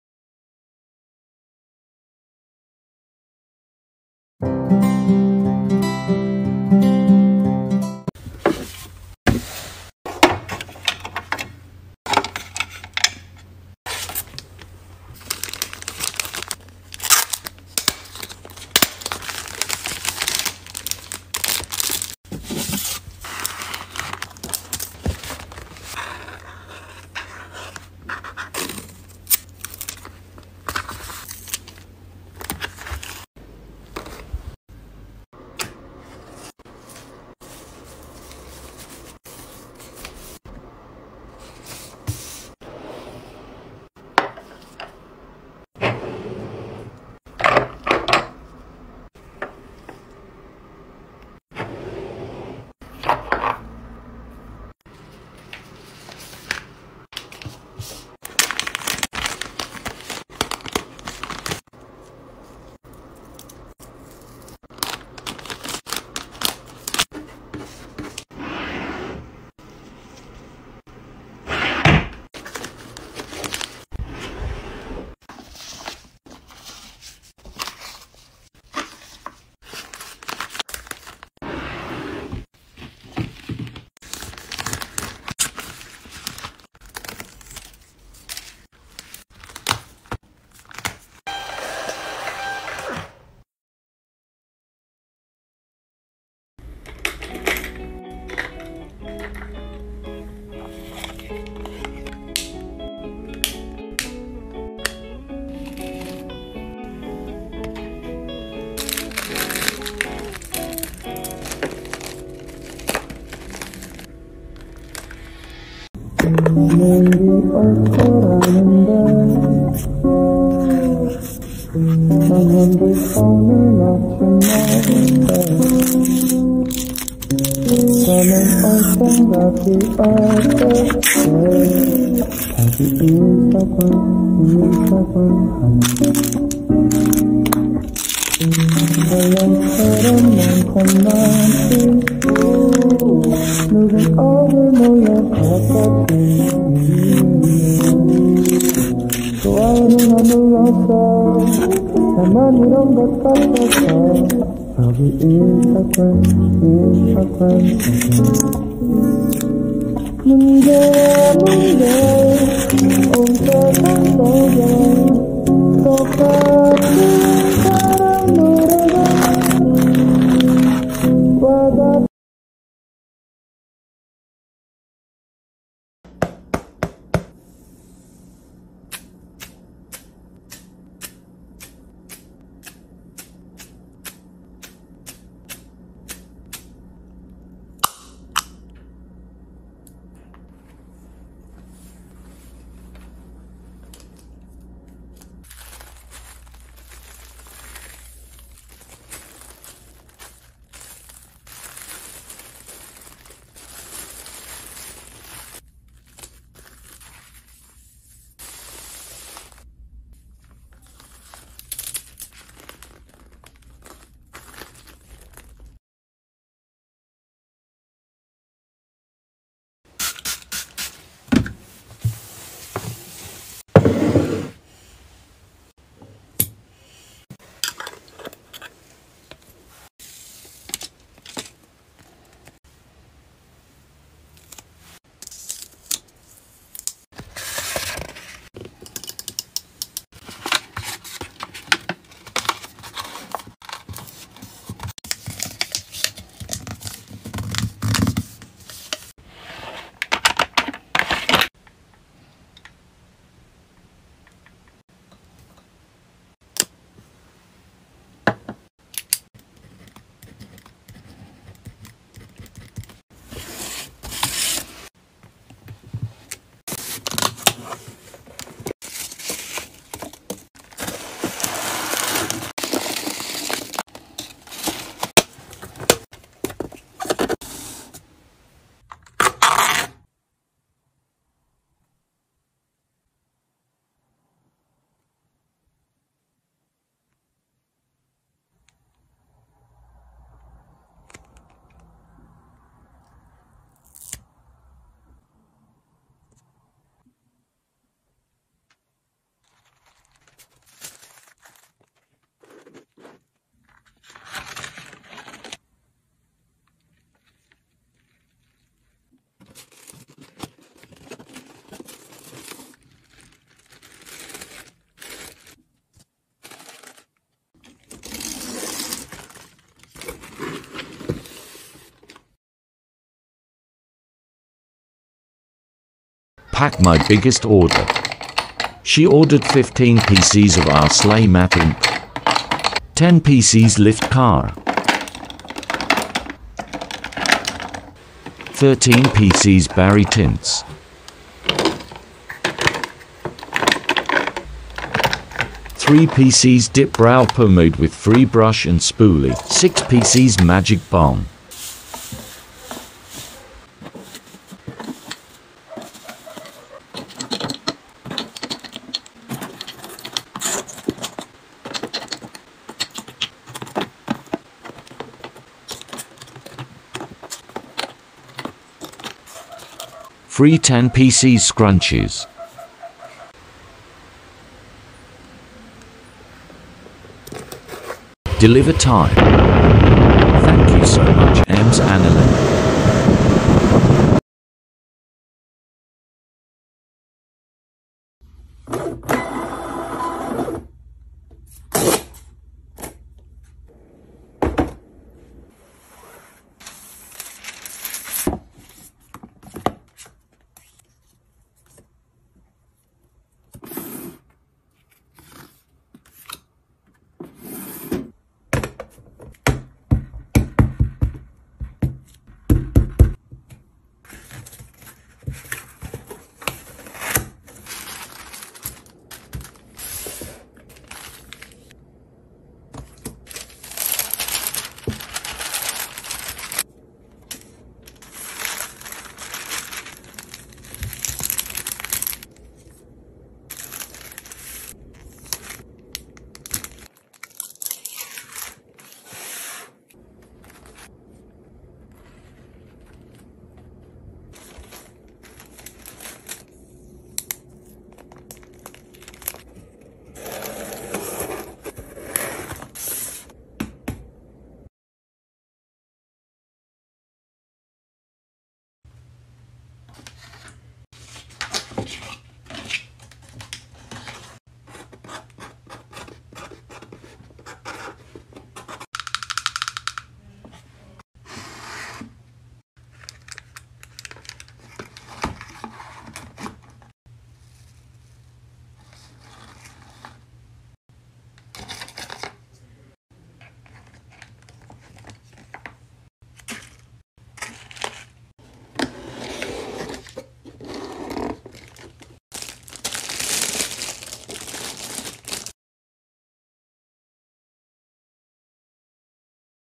Packing Orders ASMR | Tiktok Video Compilation | ASMR Collection ⭐ Part 2
This video contains a collection of packing orders ASMR clips from Tiktok. 🙂

Credits to all small business owners! 💙😘